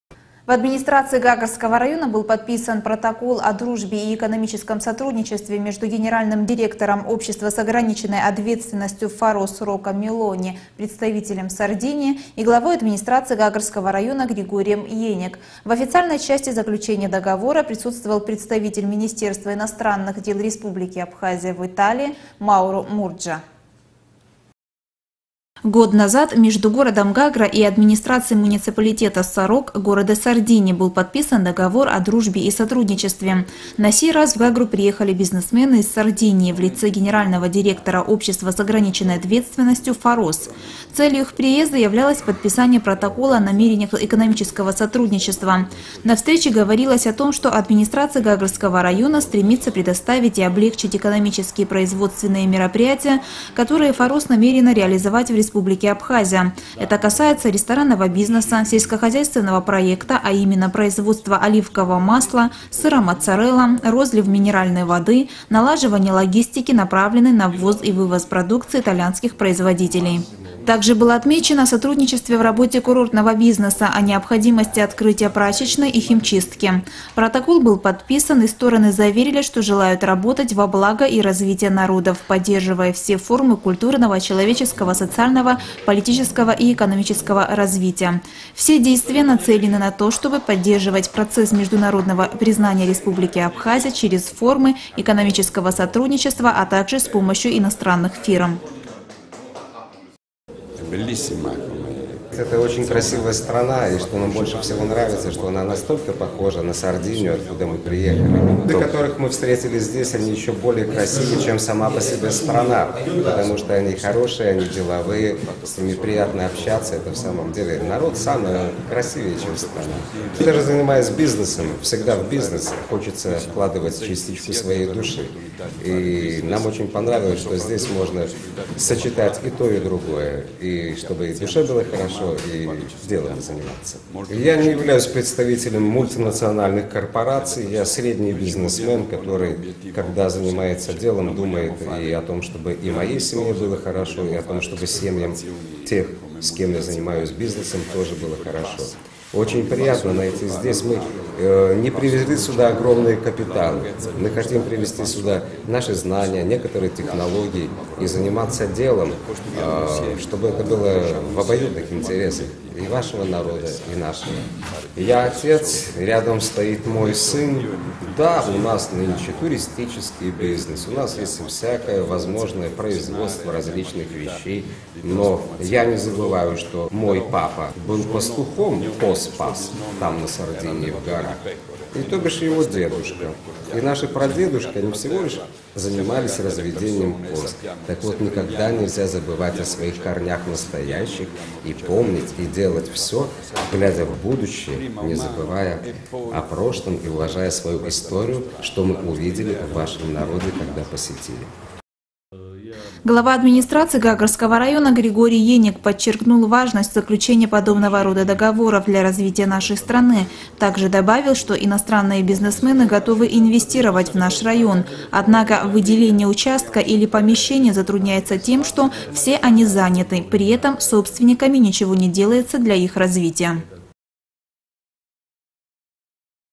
Подписание Протокола
В Администрации Гагрского района был подписан Протокол о дружбе и экономическом сотрудничестве между Генеральным директором ООО «Фарос» Рокко Мелони и Главой Администрации Гагрского района Григорием Еник.